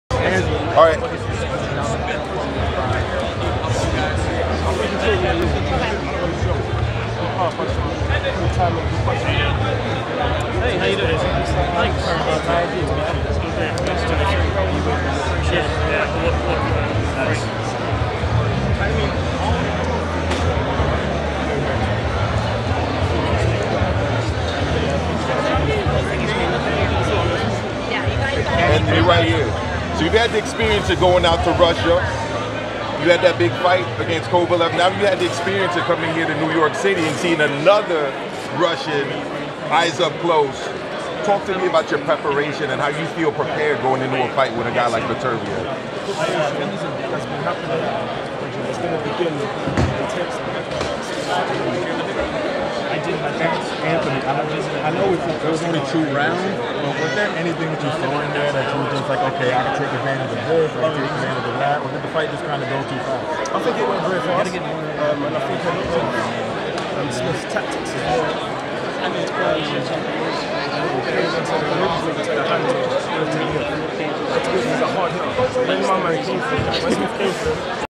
looks like Anthony Yarde is next
Anthony yarde looks like he will face Artur beterbiev in October